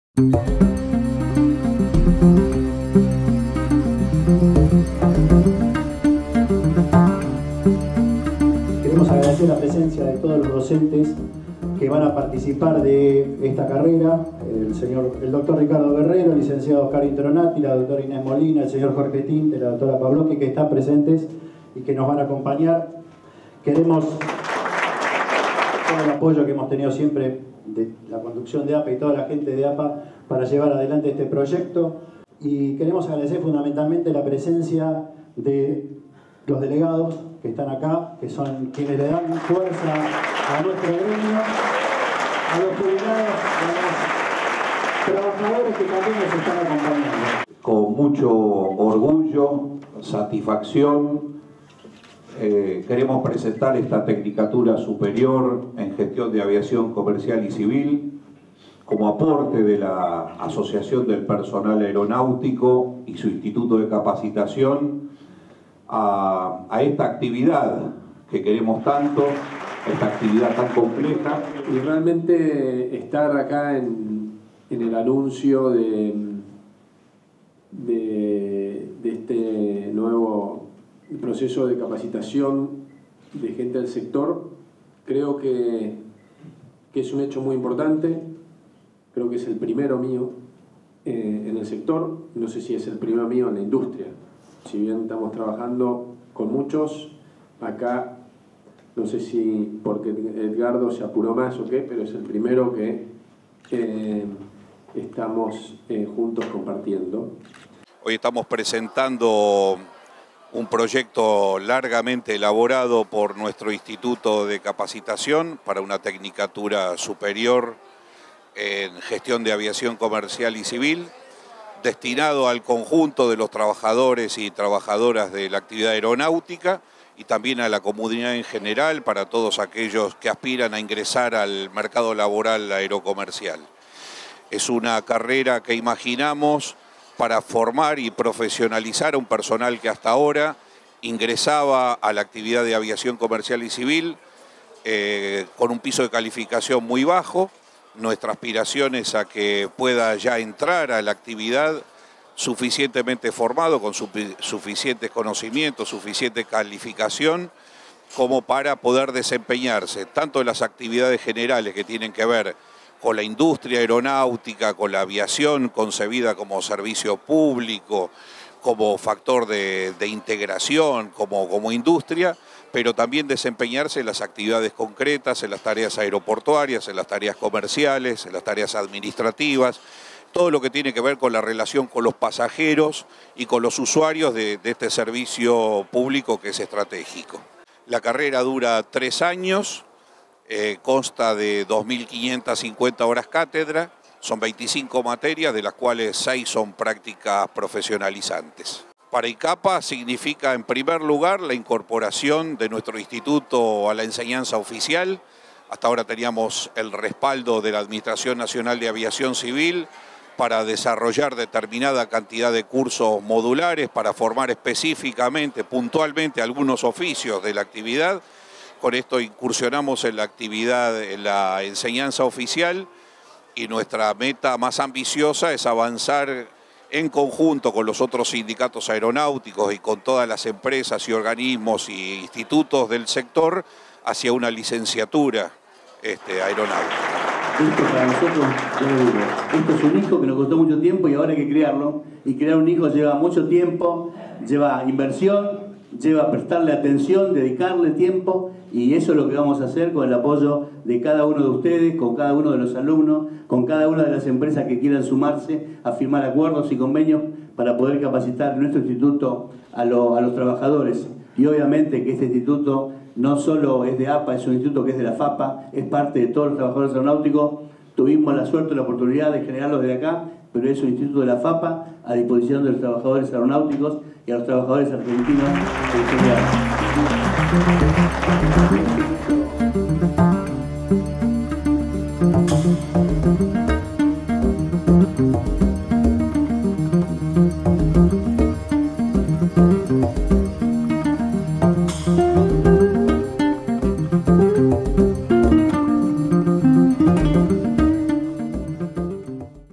APA y el ICAPA PRESENTARON LA TECNICATURA SUPERIOR EN GESTIÓN DE AVIACIÓN COMERCIAL
APA y el ICAPA PRESENTARON LA TECNICATURA SUPERIOR EN GESTIÓN DE AVIACIÓN COMERCIAL y CIVIL CON LA PRESENCIA DEL MINISTRO DE TRANSPORTE SR. GUILLERMO DIETRICH.
El Instituto de Capacitación Asociación Personal Aeronáutico (ICAPA) presentó la Tecnicatura Superior en Gestión de Aviación Comercial y Civil que comenzará a dictarse a partir de Marzo de 2017. El acto contó con la presencia del Ministro de Transporte de la Nación, Sr. Guillermo Dietrich y más de 200 invitados entre los cuales cabe destacar la presencia de representantes de las principales empresas del sector, organismos oficiales de la actividad aerocomercial, medios de prensa especializados, el plantel docente de la institución y una gran cantidad de trabajadores de la actividad. En su alocución el Director del ICAPA, Lic. Hugo Perosa, describió la trayecto del Instituto desde sus inicios y afirmó: ¨ Los egresados de la carrera estarán preparados para ingresar a una actividad que opera como un sistema en la cual es fundamental el trabajo en equipo. Una vez finalizada la misma estarán capacitados para desempeñarse en áreas de gestión y negocios, comercio aéreo, planificación de vuelos, operación de aeropuertos, empresas de rampa, atención de pasajeros y empresas, administración y contabilidad, marketing y ventas, cargas aéreas y demás trabajos relacionados con el ámbito de la aviación comercial y civil en tierra.¨ Según las palabras de Edgardo Llano, Secretario General de APA (Asociación Personal Aeronáutico), el objetivo fundamental de esta tecnicatura es el de llenar el vacío en materia de formación profesional aeronáutica existente con el propósito de convertirse en el nexo entre el lugar de trabajo y el conocimiento específico y que la misma había sido pensada como un aporte desde APA a la profesionalización y desarrollo de la actividad aerocomercial argentina. Finalmente el Ministro de Transporte Guillermo Dietrich felicitó a APA por este logro y destacó “la importancia de trabajar en conjunto desde diferentes sectores en pos del objetivo común de desarrollar el mercado y la industria del transporte aereo".